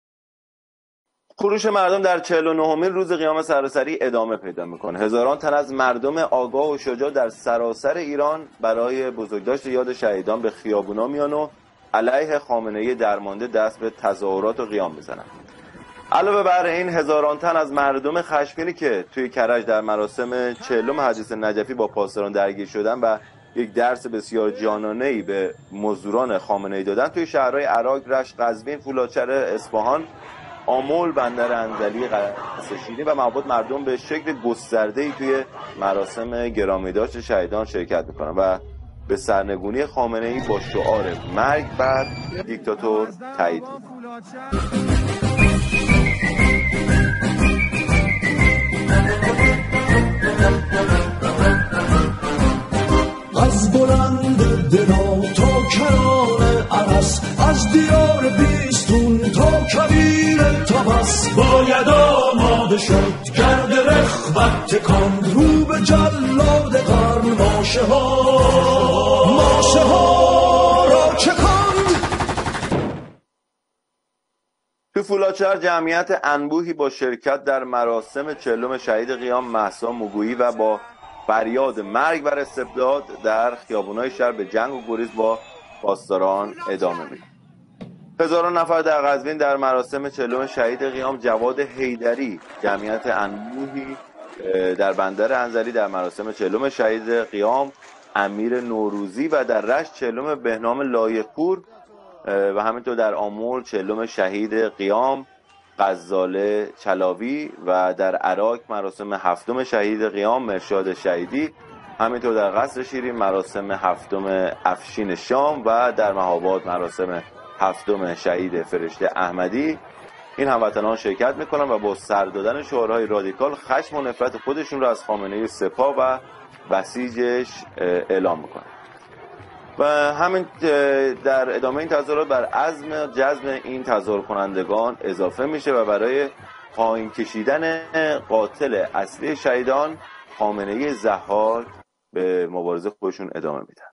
چهلم شهدای قیام سیلی آتشین بر بناگوش خامنه ای
در چهل و نهمین روز قیام سراسری ، هزاران تن از مردم آگاه و شجاع  در سراسر ایران برای بزرگداشت یاد شهیدان به خیابانها آمدند وعلیه خامنه‌ای درمانده  دست به تظاهرات  و قیام زدند